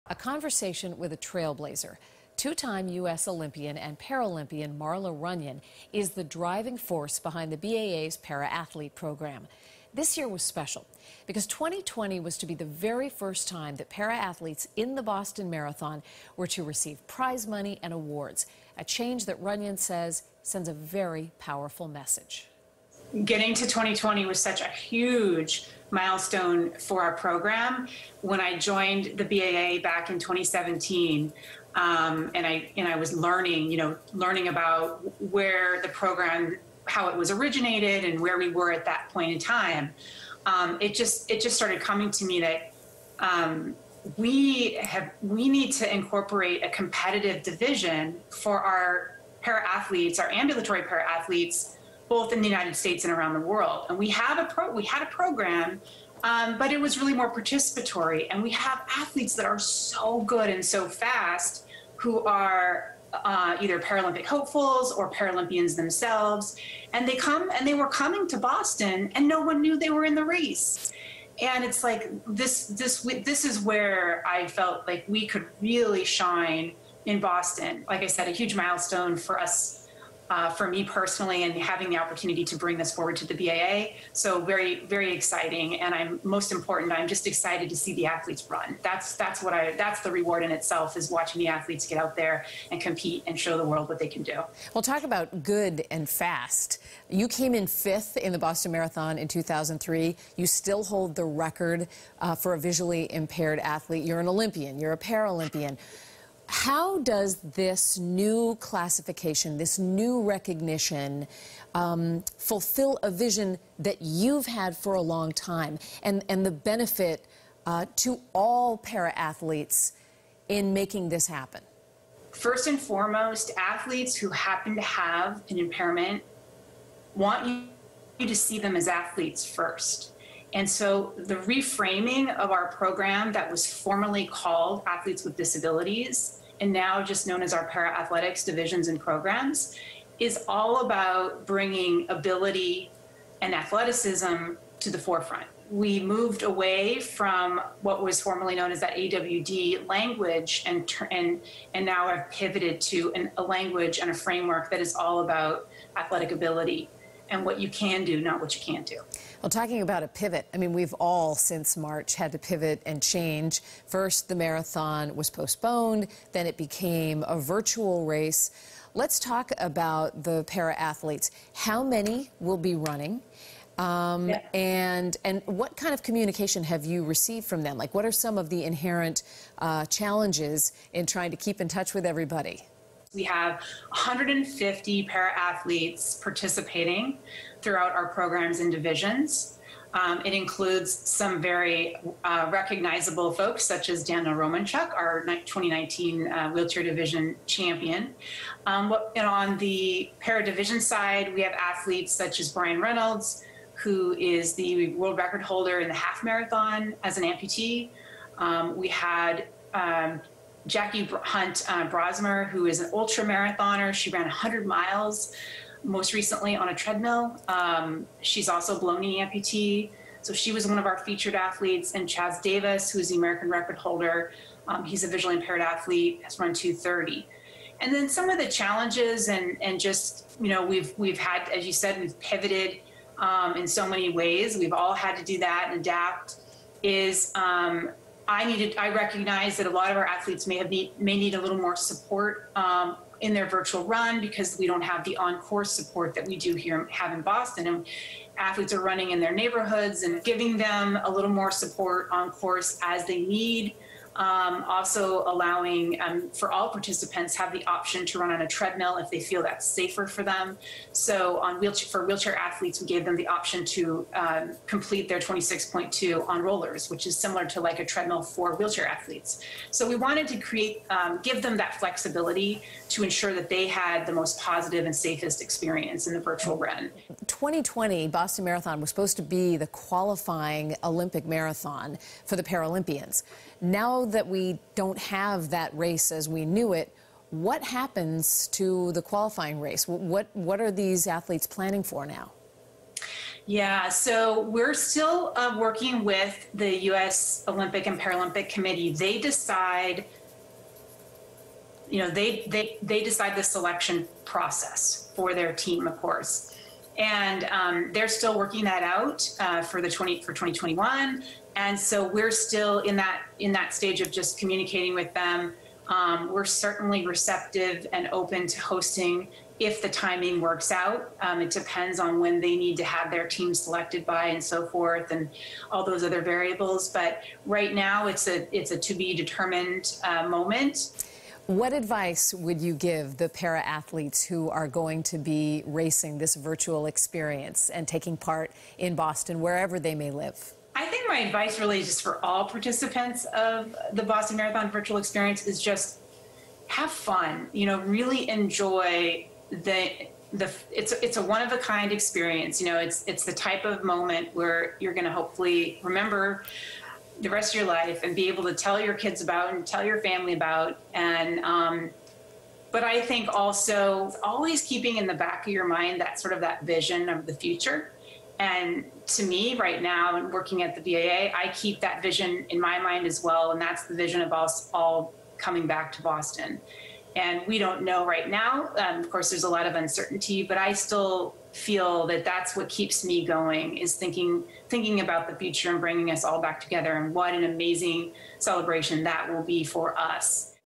A Conversation With 2-Time Olympian Marla Runyan, BAA Para Athletes Manager
Runyan pushed for a competitive division for ambulatory para athletes in the Boston Marathon.